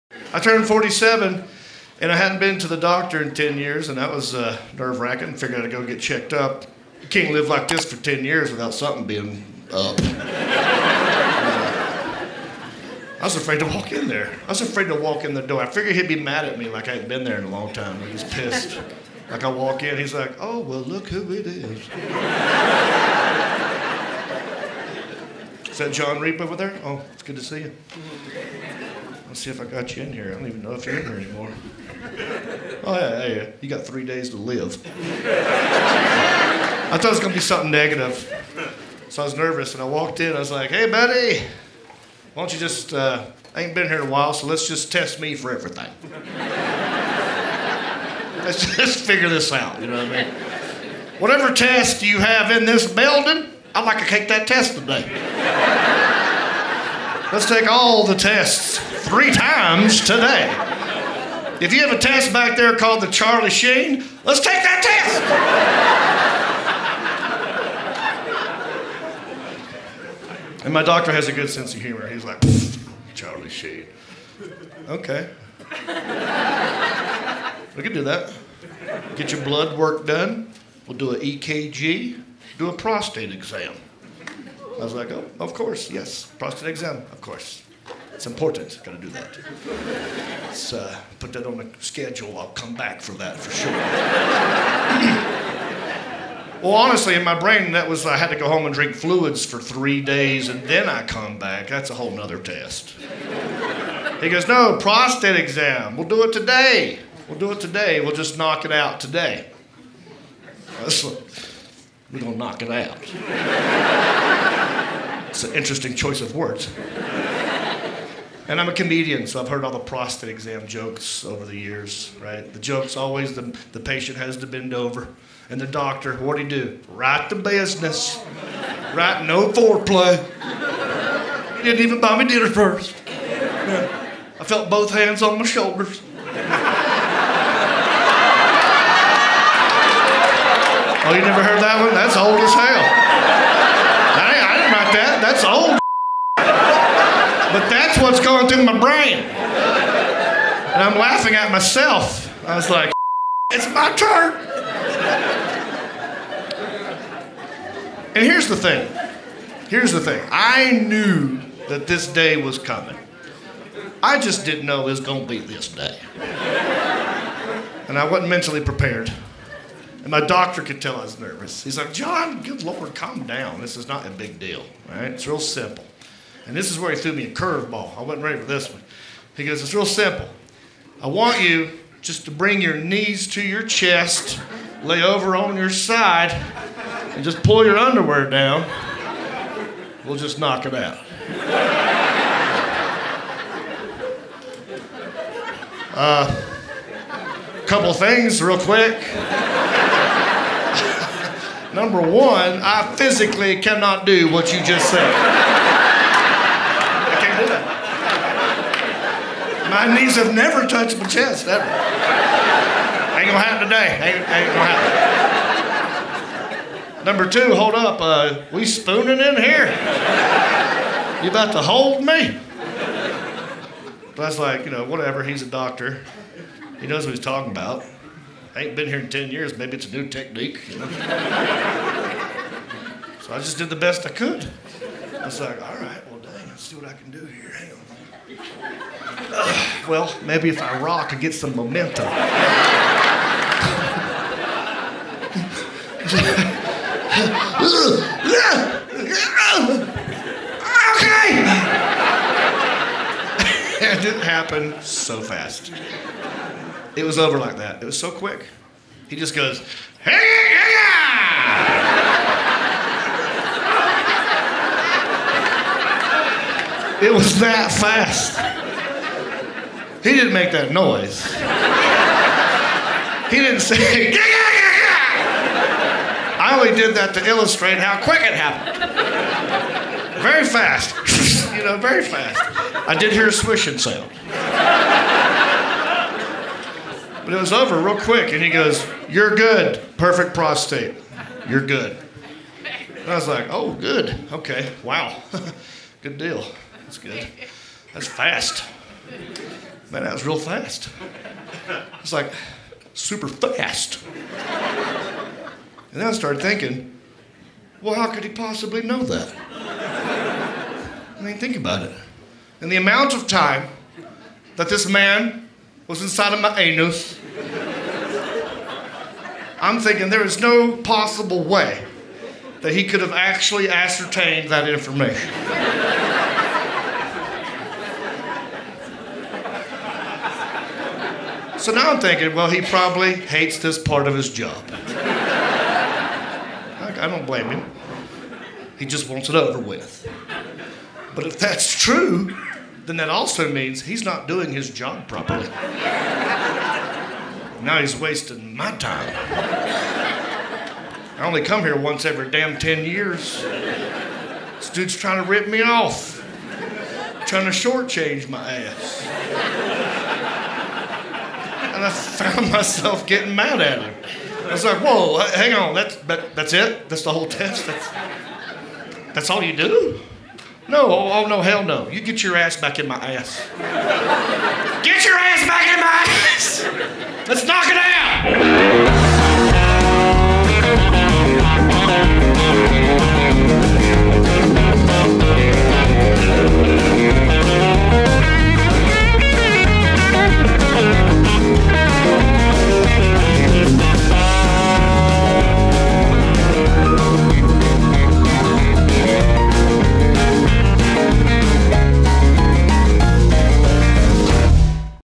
When Your Doctor Wants to Do a Prostate Exam | Jon Reep
Comedian Brad Stine on when your doctor wants to do a prostate exam...

More videos for you to check out...

Does Jon have COVID? Manny Blu, Residual Checks & Russian Letters - Country-ish with Jon Reep Ep. 33